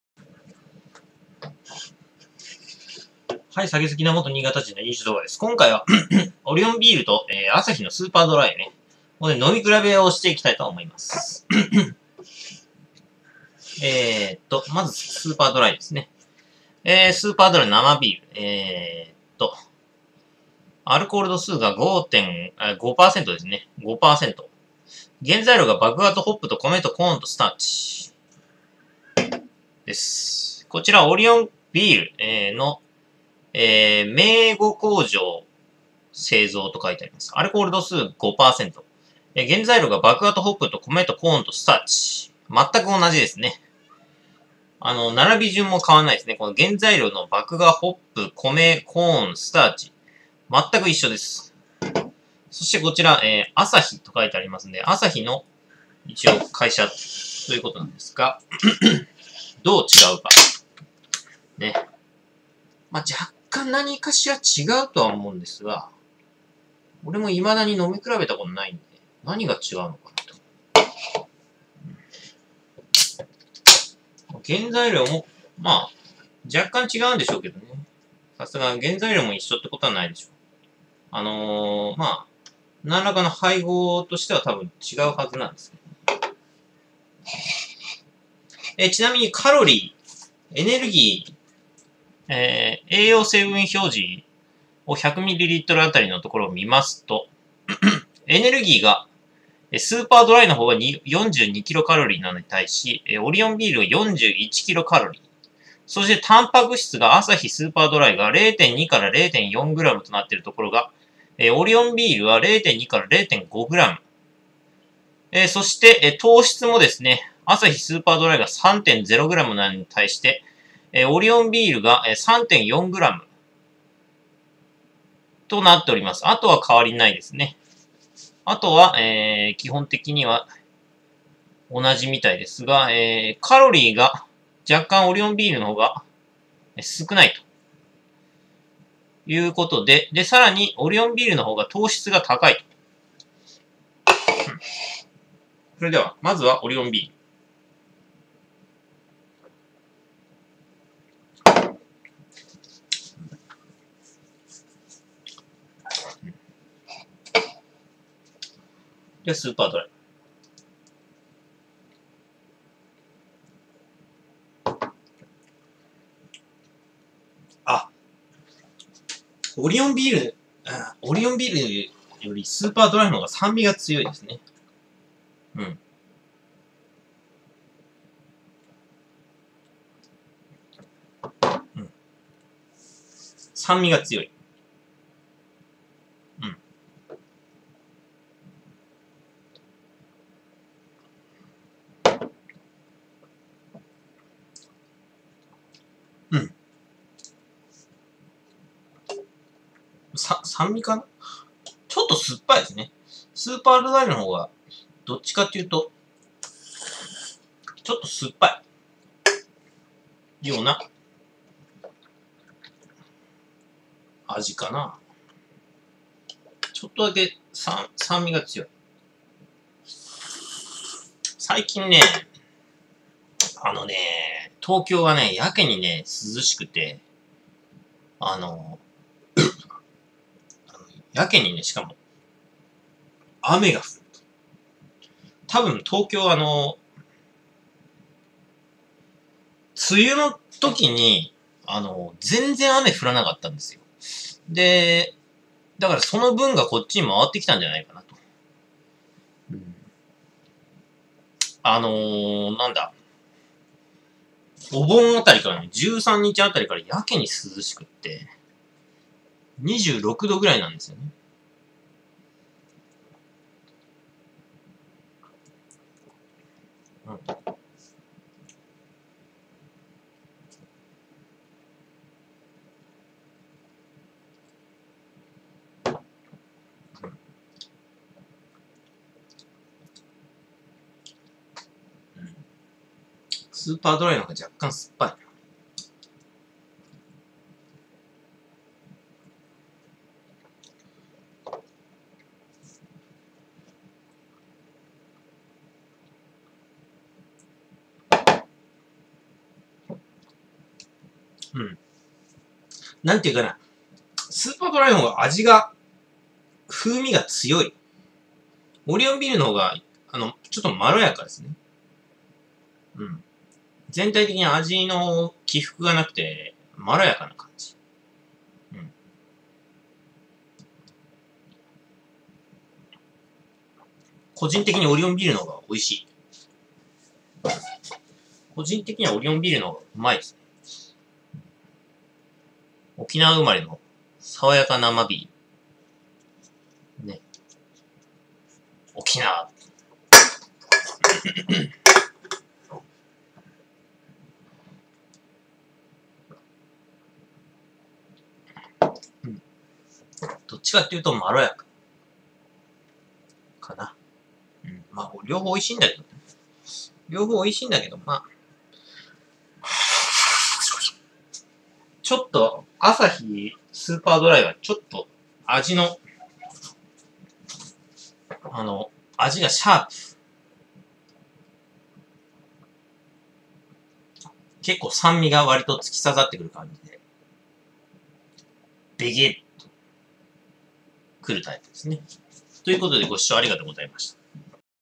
酒好きな元新潟人の飲酒動画 part1351 オリオンビールとスーパードライを呑み比べる
LINEもやってます。良ければ申請してね⇒